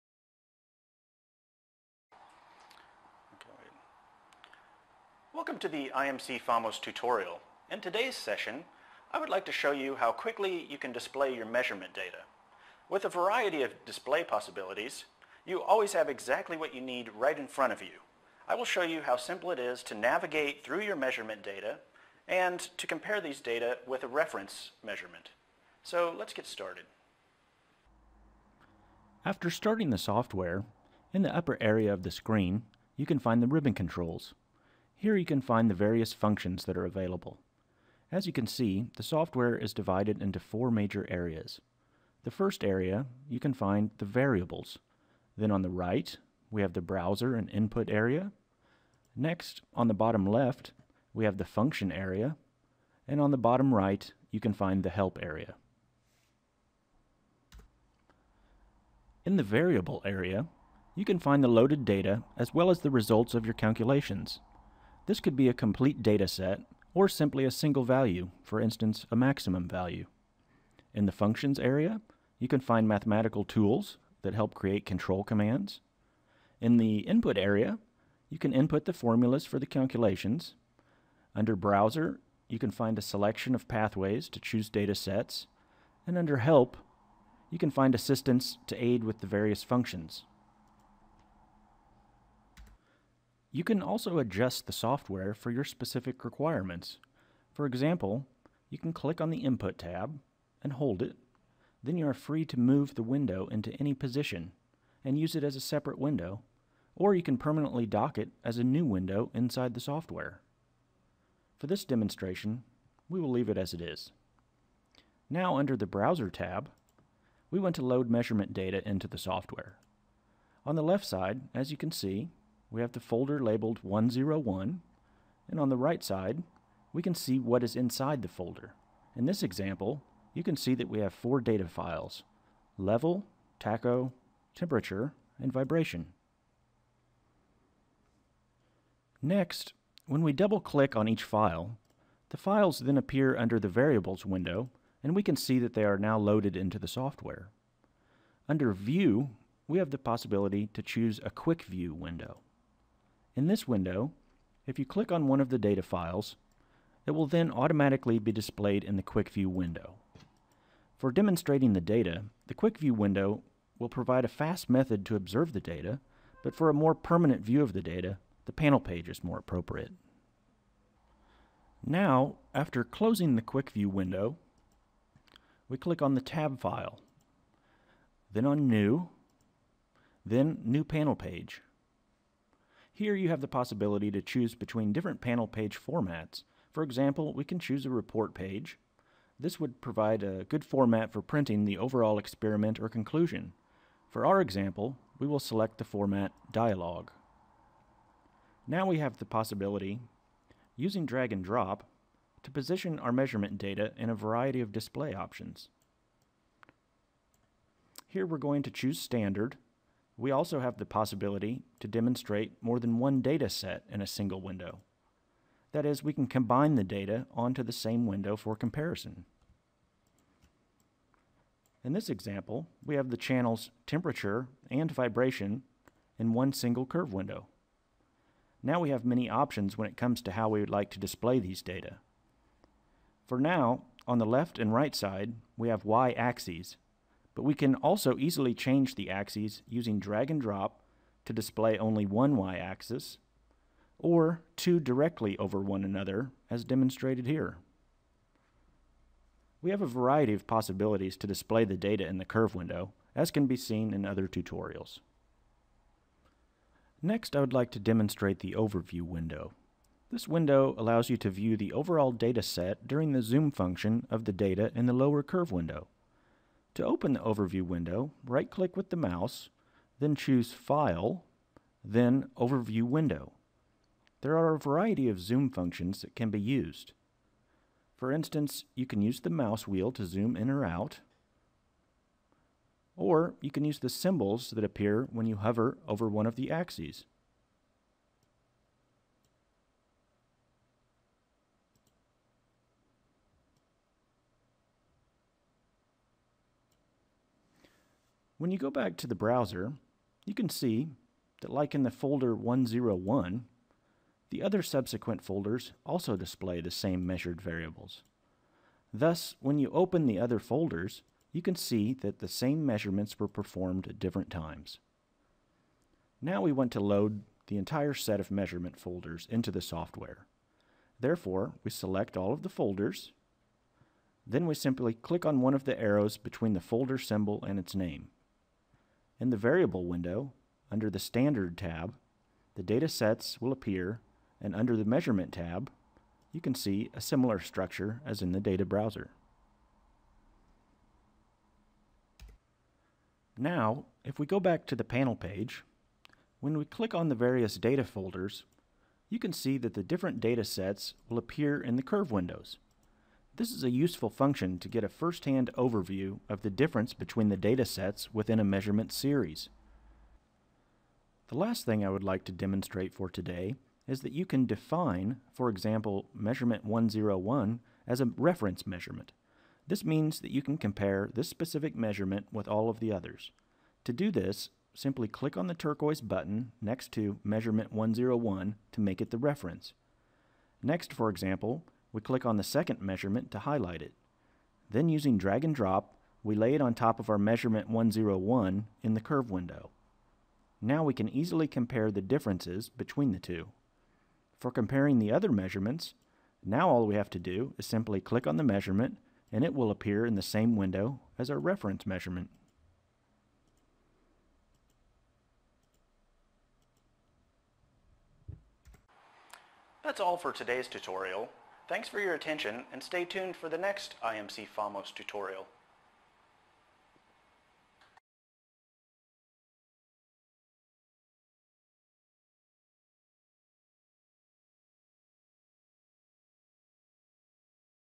FAMOS 7 Data Viewing and Comparing
This tutorial gives you a short introduction to the graphical user interface from imc FAMOS and a short introduction to the imc Databrowser